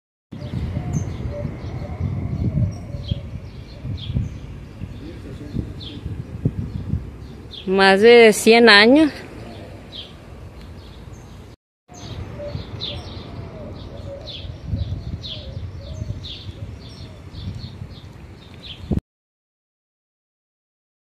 Tumba muy antigua de una colonia china (Año 1899). Más de 100 años de antigüedad.
En esta tumba muy antigua descansa en paz una colonia china, del año 1899. Panteón San Juan, en Culiacán.